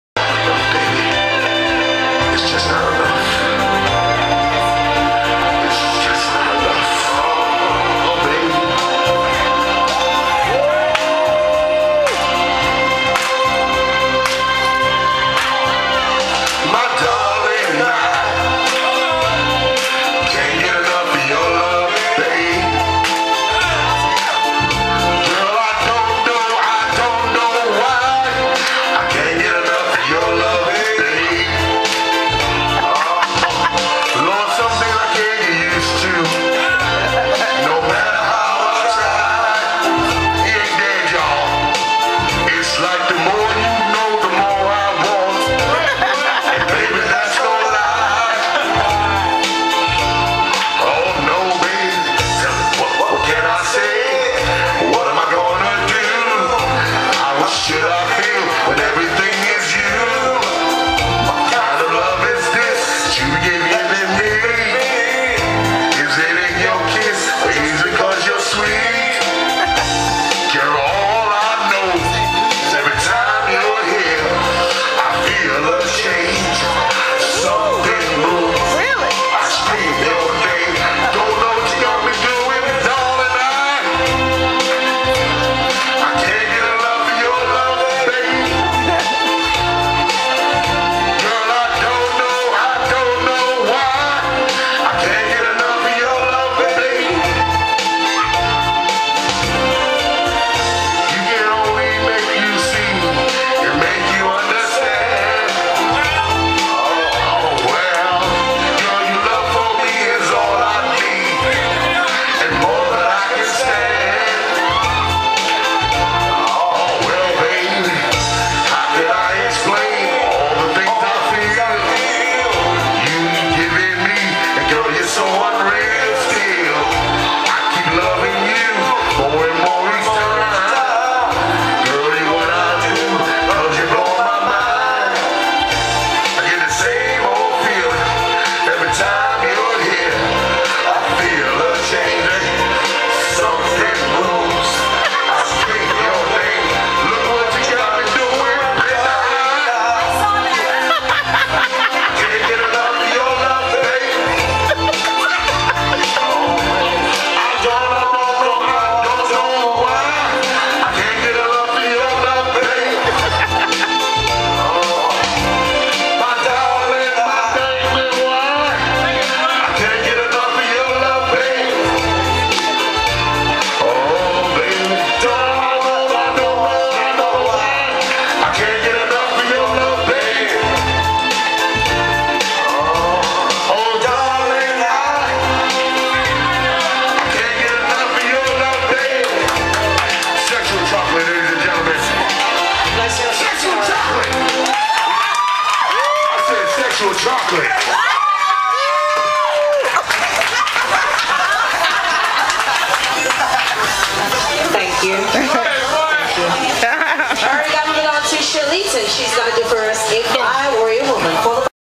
Barry White Tribute.wmv
Derrick singing Barry White